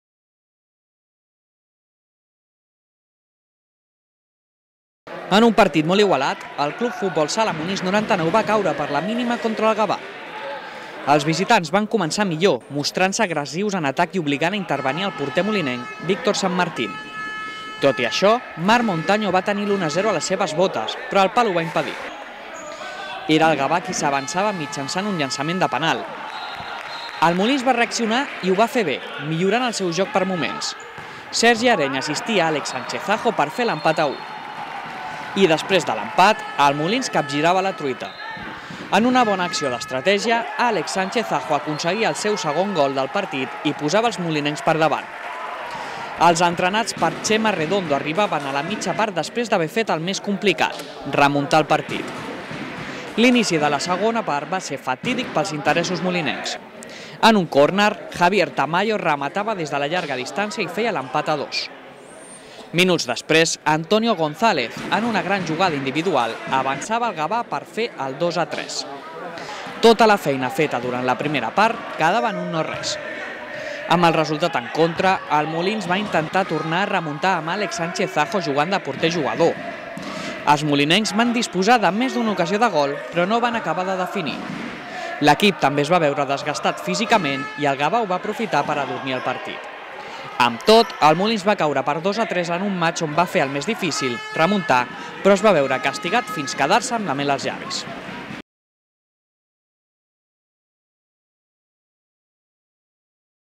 CFS Molins 99 - Gavà novembre 2015
Resum del partit entre el CFS Molins 99 i el FS Gavà de Tercera Nacional de futbol sala.